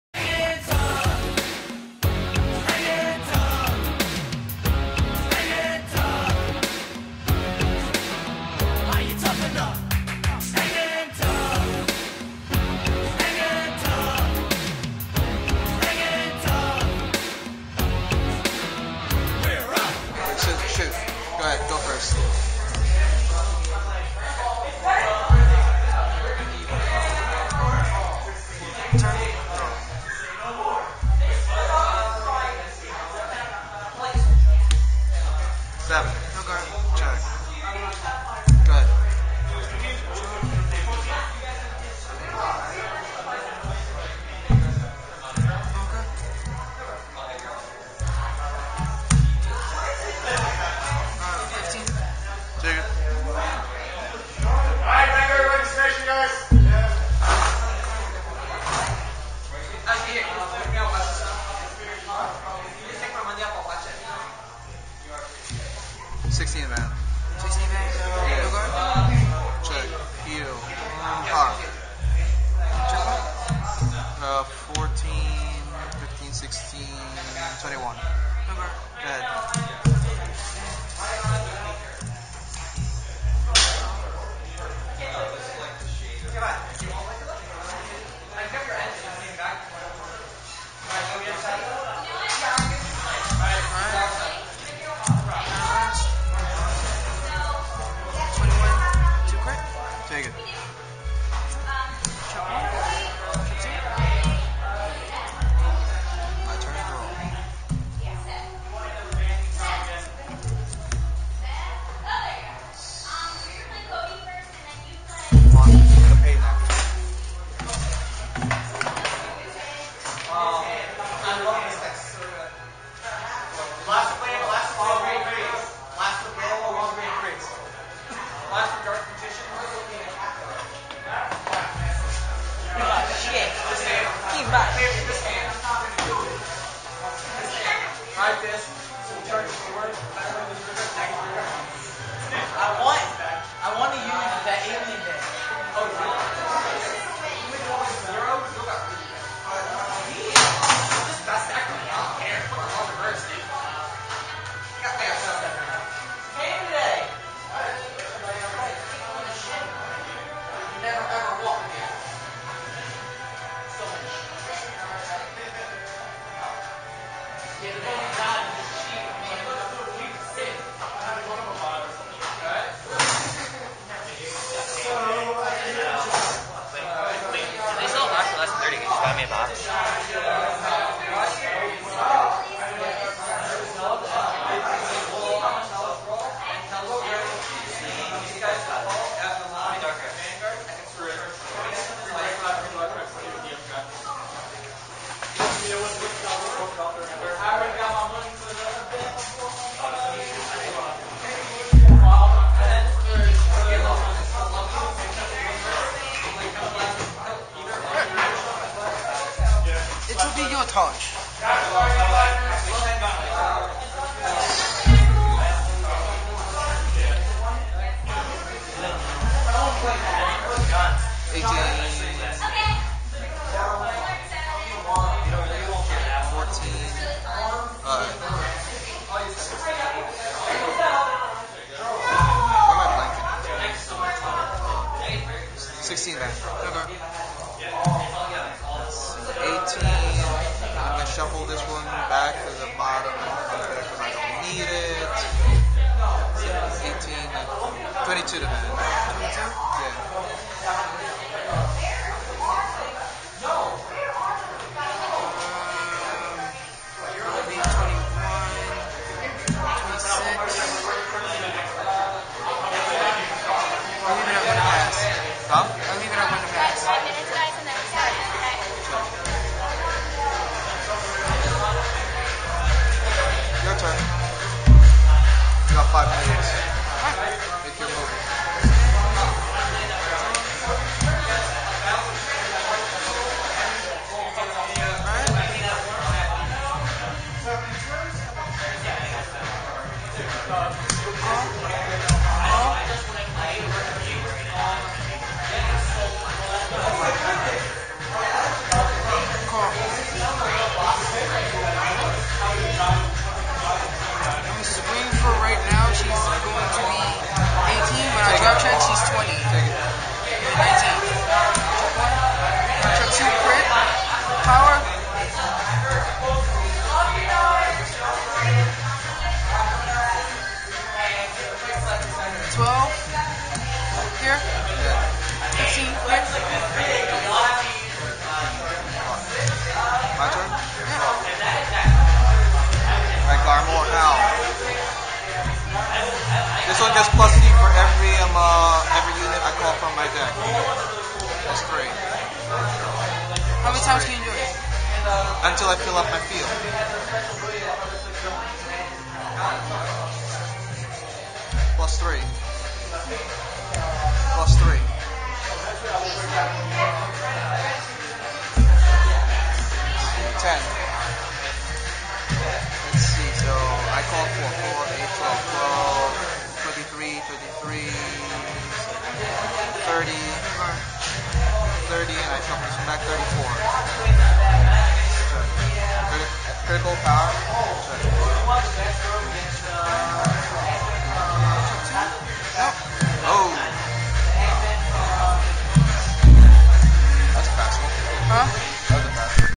Garmore/Alfred vs Hexagonal/Pentagonal Magus
Gold versus Magus! Friendship over prediction? Let's go!

Filmed at Adventureland in the Voorhees Town Center.